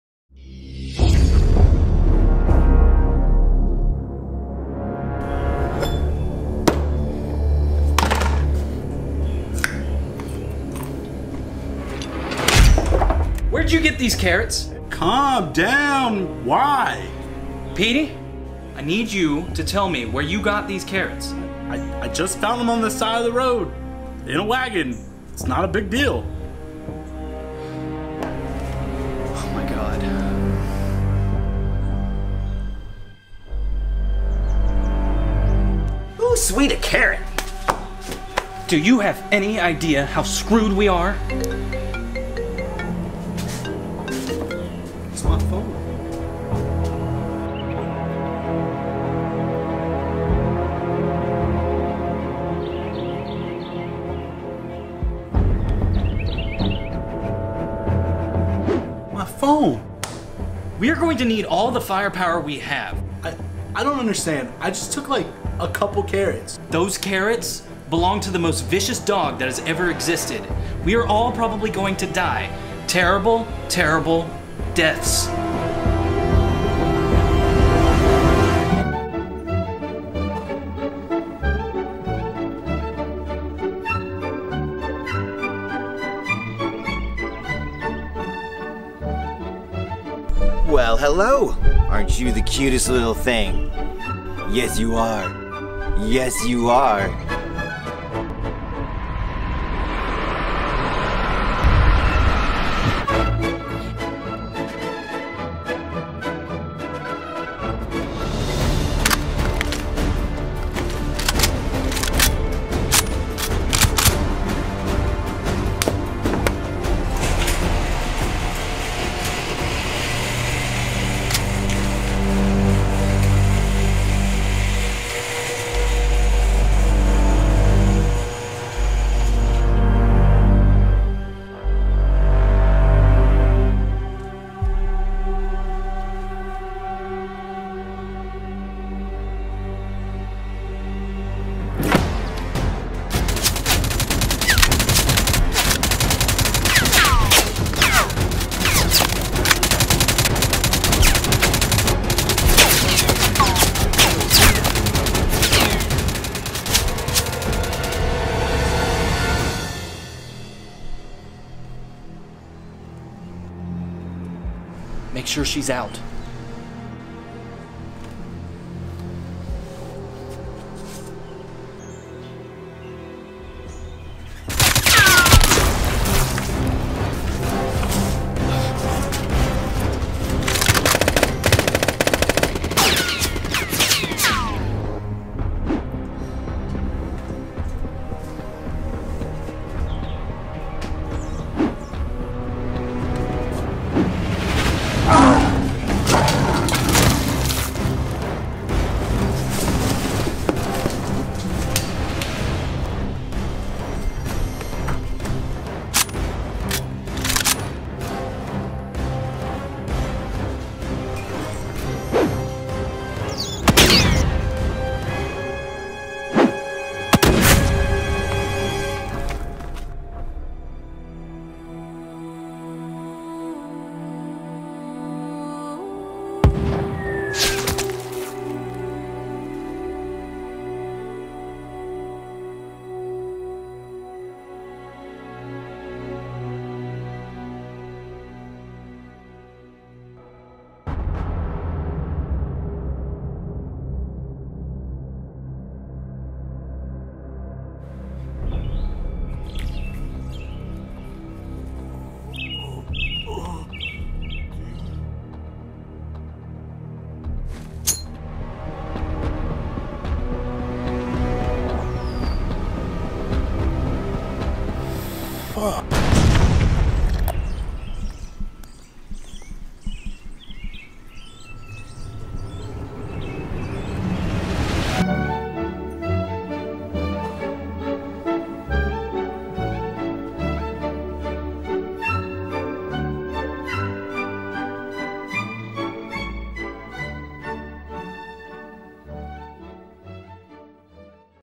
Winnie!
Guns. Chaos. Carrots. (in 4k)

Despite having to clean up a ton of fake blood, this was incredibly fun to film.
(absolutely NO animals were harmed in the making of this short)

Love ya goodnight!